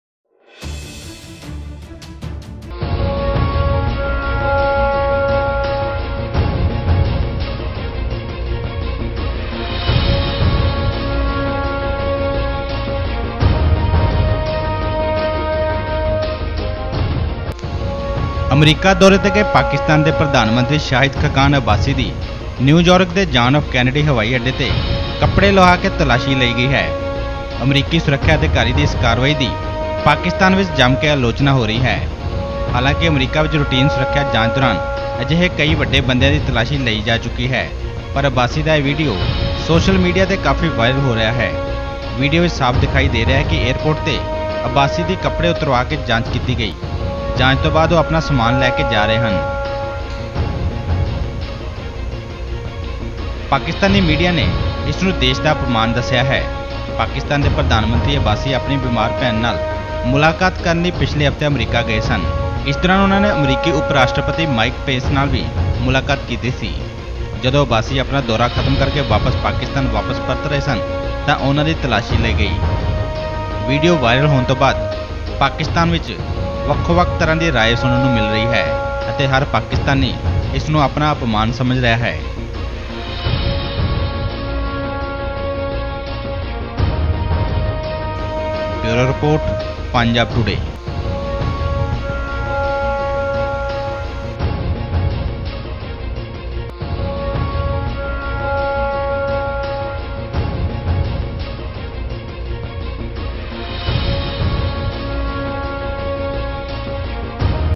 ਅਮਰੀਕਾ ਨੇ ਲੁਹਾਏ ਪਾਕਿਸਤਾਨ ਪ੍ਰਧਾਨ ਮੰਤਰੀ ਦੇ ਕੱਪੜੇ,Pakistan PM Viral Video On US Airport
ਅਮਰੀਕਾ ਨੇ ਲੁਹਾਏ ਪਾਕਿਸਤਾਨ ਪ੍ਰਧਾਨ ਮੰਤਰੀ ਦੇ ਕੱਪੜੇ
ਪਾਕਿਸਤਾਨੀ ਲੋਕਾਂ ਤੇ ਮੀਡੀਆ ਚ ਗੁੱਸਾ ,ਕਿਹਾ ਅਮਰੀਕਾ ਨੇ ਕੀਤਾ ਪਾਕਿਸਤਾਨ ਦਾ ਅਪਮਾਨ